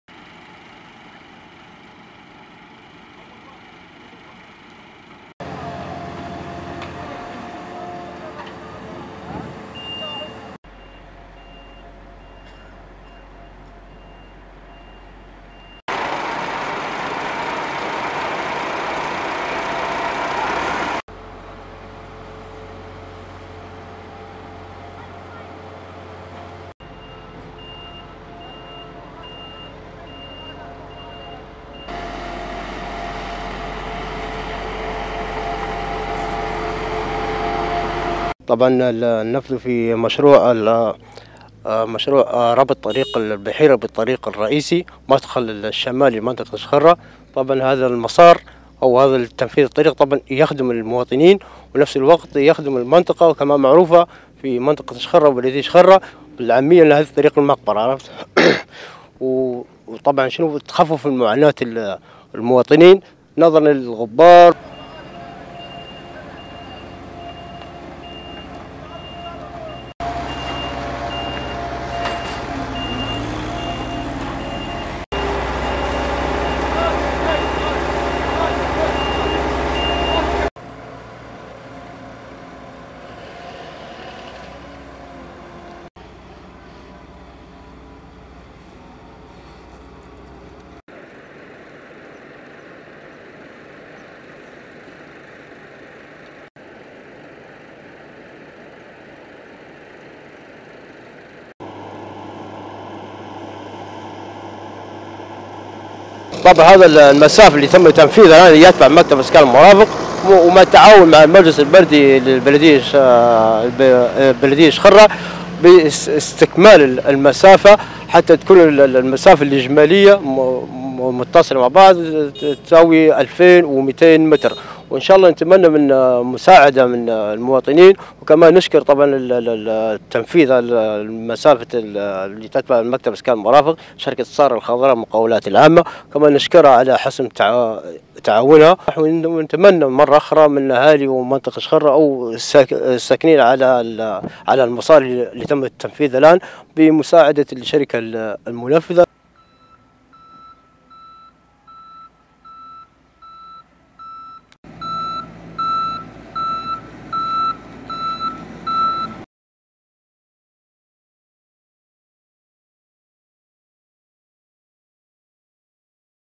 مكتب الإسكان والمرافق ببلدية إجخرة يشرع في رصف طريق المدخل الشمالي للبلدية LCNA
وكالة الغيمة الليبية للأخبار - إجخرة.
شرع مكتب الإسكان والمرافق وبالتعاون مع المجلس التسييري لبلدية إجخرة في تنفيذ مشروع رصف طريق المدخل الشمالي للبلدية بطول (2200) متر، وانطلقت المرحلة الأولى من تنفيذ المشروع بعد توفر مادة (الأسفلت) برصف (600) متر، والتي يشرف عليها مكتب الإسكان والمرافق وستليها المرحلة الثانية برصف (1600) متر، والتي يشرف علي تنفيذها المجلس التسييري لبلدية إجخرة.